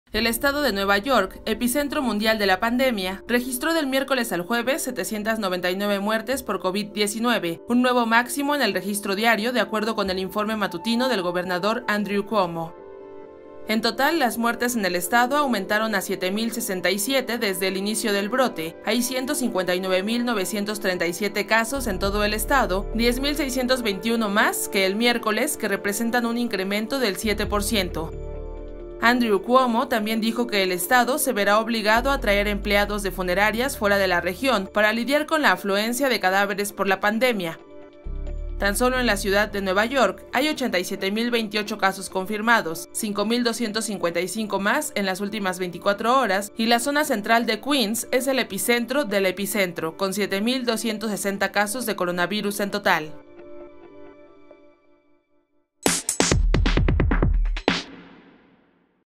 NY alcanza nuevo récord de 799 muertes en un día
El estado de Nueva York, epicentro mundial de la pandemia, registró del miércoles al jueves 799 muertes por COVID-19, un nuevo máximo en el registro diario, de acuerdo con el informe matutino del gobernador, Andrew Cuomo.